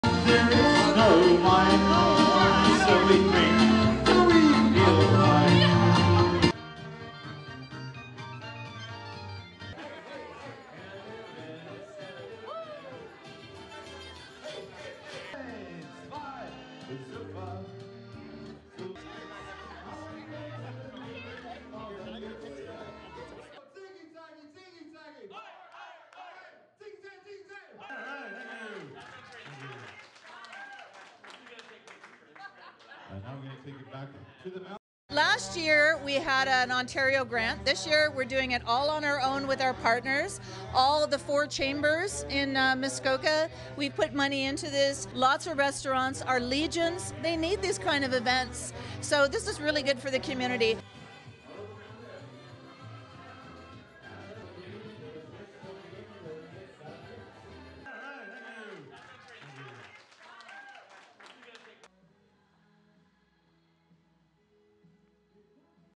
GERMAN BAND - HappyWanderers1972 (Toronto German Band)
HAPPY WANDERER'S Live at MUSKOKA OKTOBERFEST 2022 celebration...(Toronto German Band / Bavarian Band / Polka Band / Omp Pah Pah band / Oktoberfest Band / Beer Band / German Band / Bier Band / happywanderers1972)